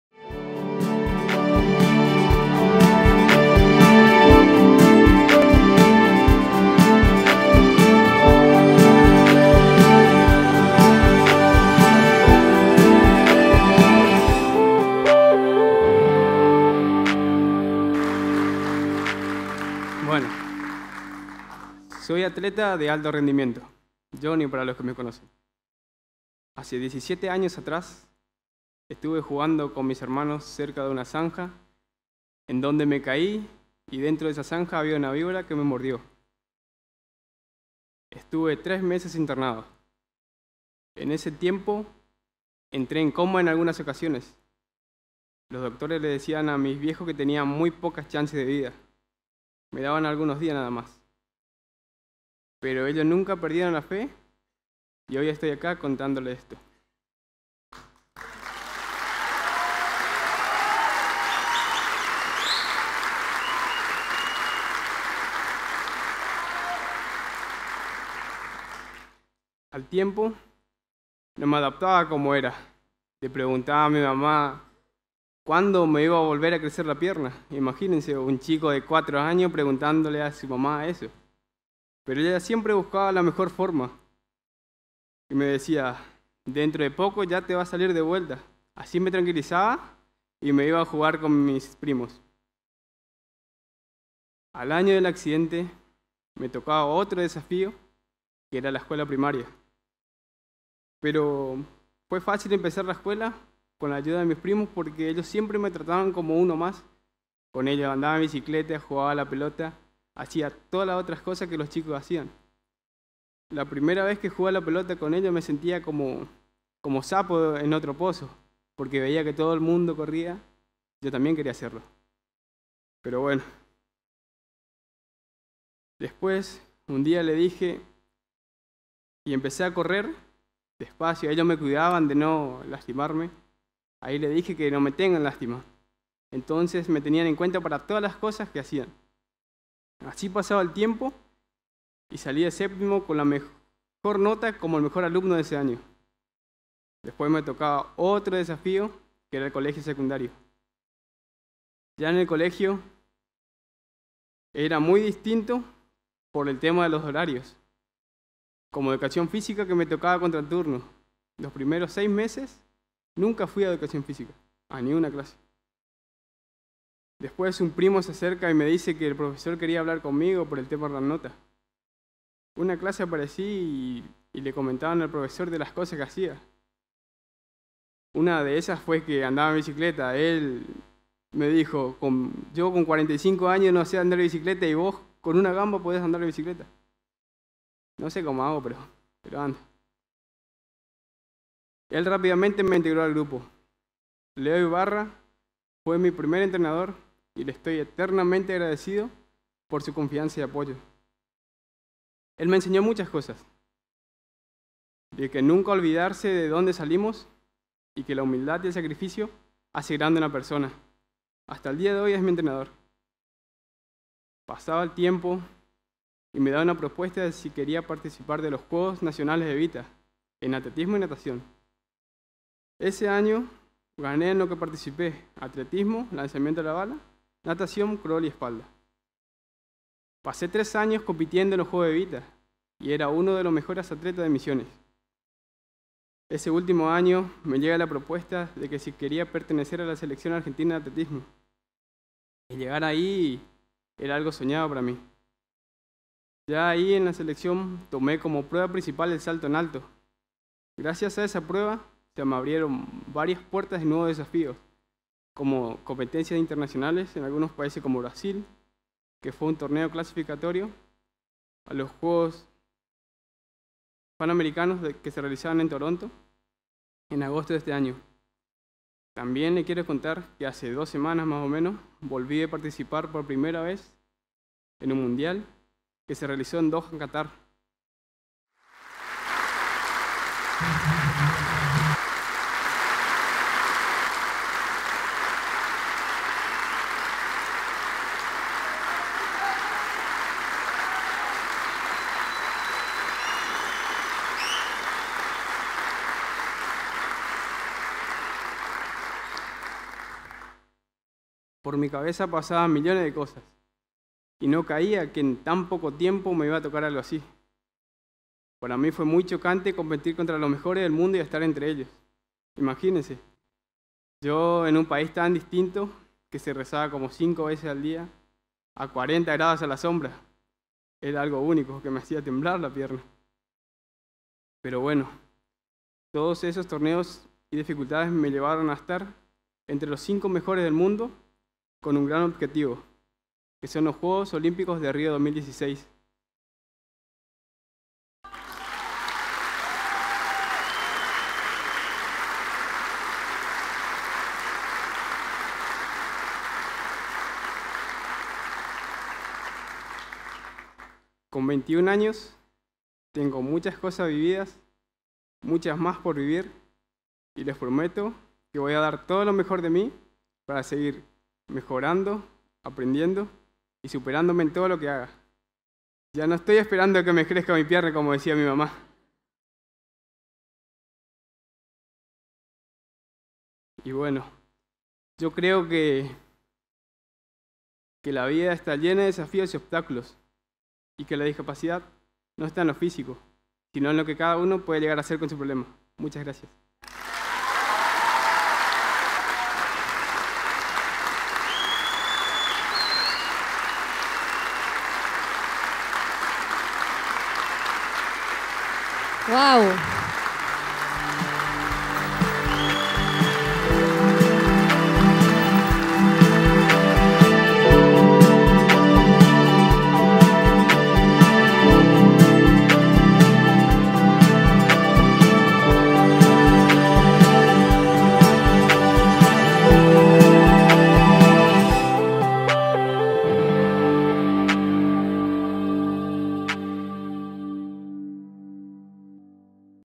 VAMOSAZOOMAR - YONI AVELLANEDA - Sin límites
Jonathan es un atleta misionero -oriundo de Eldorado- a quien le amputaron una pierna tras ser picado por una yarará en pleno monte.
Miembro de la Selección Argentina de Atletismo adaptado con especialidad en la prueba de salto en alto, se encuentra entre los primeros 10 mejores atletas del mundo en su disciplina.